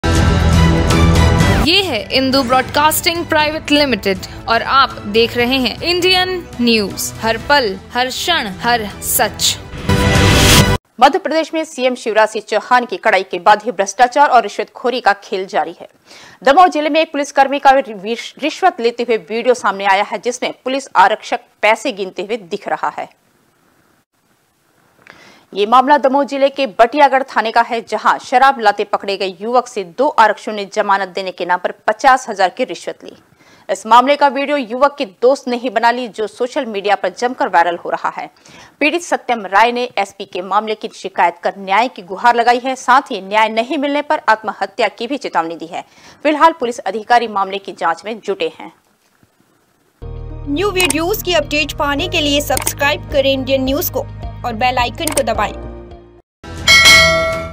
दमोह : शराब लाते युवक को पुलिस ने धमकाकर मांगी, रिश्वत
Indian News :  मध्यप्रदेश में सीएम शिवराज सिंह चौहान की कड़ाई के बाद भी भ्रष्टाचार और रिश्वतखोरी का खेल जारी है। दमोह जिले में एक पुलिसकर्मी का रिश्वत लेते हुए वीडियो सामने आया है, जिसमें पुलिस आरक्षक पैसे गिनते हुए दिख रहा है।
ये मामला दमोह जिले के बटियागढ़ थाना का है। जहां शराब लाते पकड़े गए युवक से दो आरक्षकों ने जमानत देने के नाम पर पचास हजार की रिश्वत ली। इस मामले का वीडियो युवक के दोस्त ने बना ली, जो सोशल मीडिया पर जमकर वायरल हो रहा है। पीड़ित सत्यम राय ने एसपी से मामले की शिकायत कर न्याय की गुहार लगाई है। साथ ही न्याय नहीं मिलने पर आत्महत्या की भी चेतावनी दी है। फिलहाल पुलिस अधिकारी मामले की जांच में जुटे हैं।

Our Links